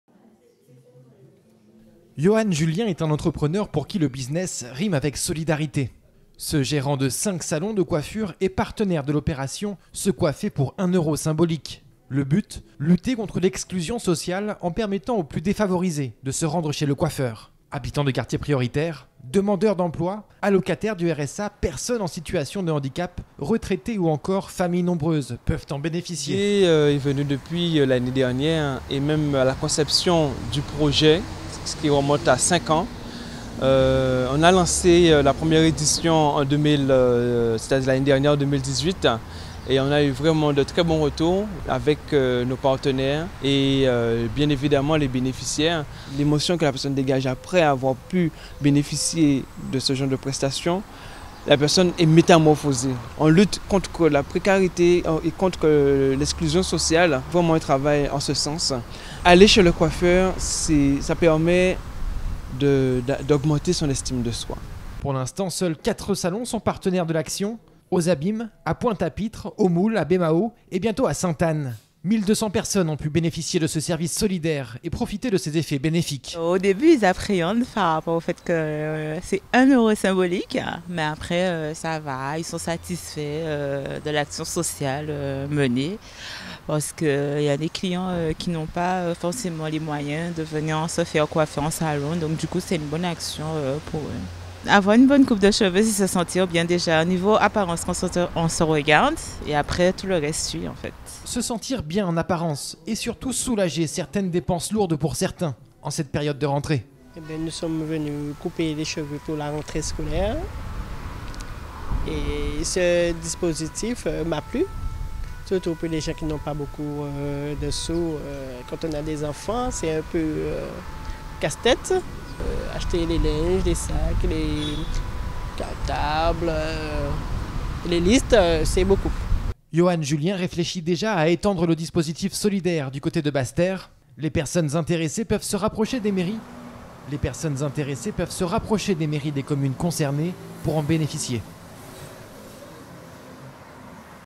🔴 Ce coiffeur propose des coupes de cheveux à 1 euros pour lutter contre l'exclusion sociale ❤️
"L'émotion de la personne qui en bénéficie est énorme à chaque prestation, la personne est métamorphosée"

Se coiffer pour 1 euro symbolique: un dispositif de lutte contre la précarité ⤵️